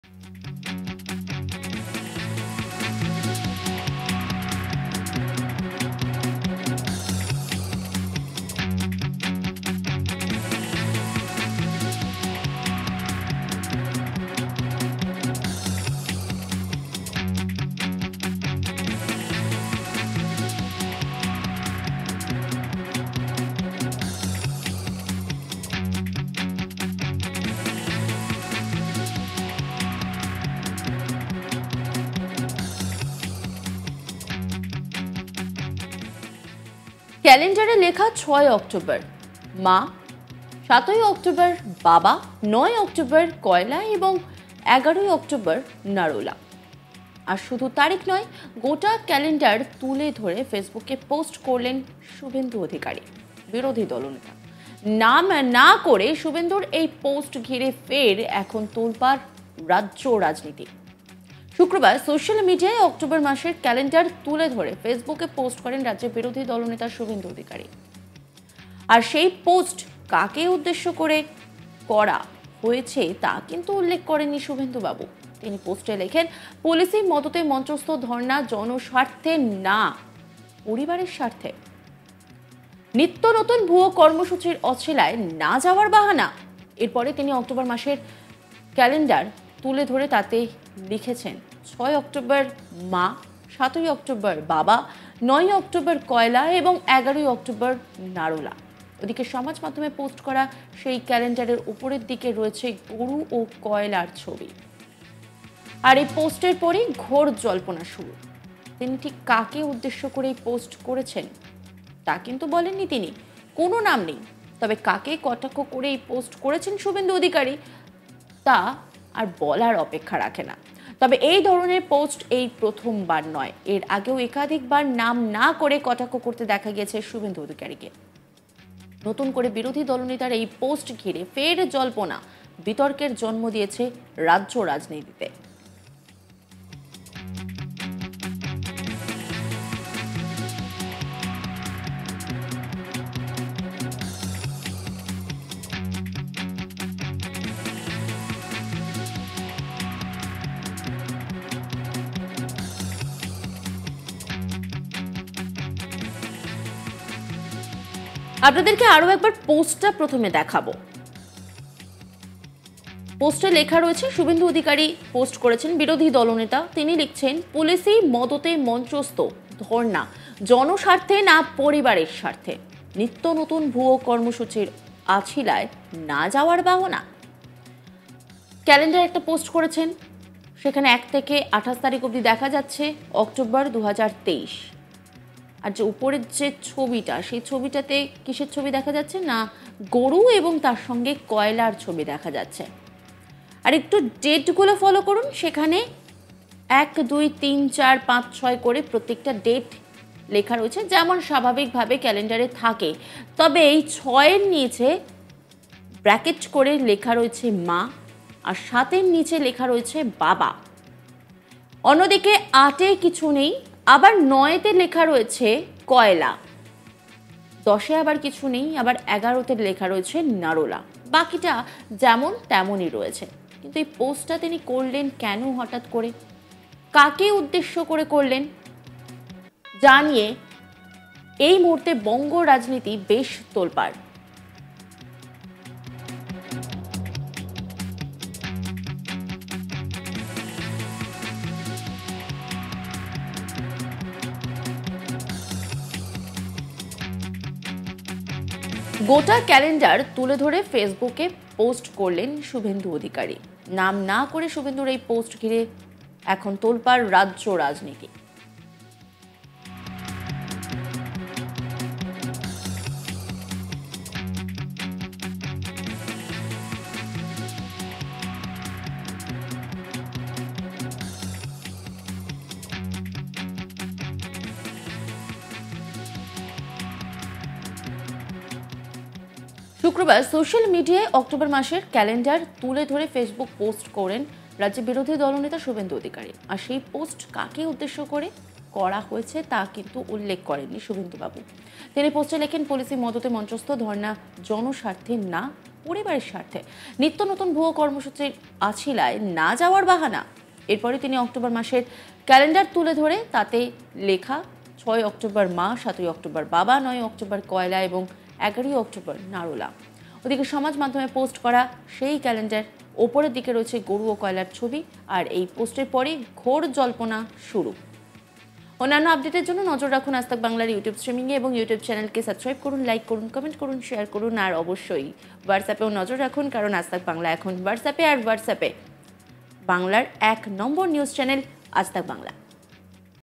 9 ‘কয়লা’, 11 ‘নারোলা’! শুভেন্দুর ক্যালেন্ডার পোস্টে কী ইঙ্গিত? Suvendu Adhikari On ED Summon
ক্যালেন্ডারে লেখা 6 অক্টোবর মা, 7 অক্টোবর বাবা, 9 অক্টোবর কয়লা এবং 11 অক্টোবর নারোলা। আর শুধু তারিখ নয়। গোটা ক্যালেন্ডার তুলে ধরে ফেসবুকে পোস্ট করলেন শুভেন্দু। নাম না করে শুভেন্দুর এই পোস্ট ঘিরে ফের এখন তোলপার রাজ্য রাজনীতি।

9 ‘কয়লা’, 11 ‘নারোলা’! শুভেন্দুর ক্যালেন্ডার পোস্টে কী ইঙ্গিত? Suvendu Adhikari On ED Summon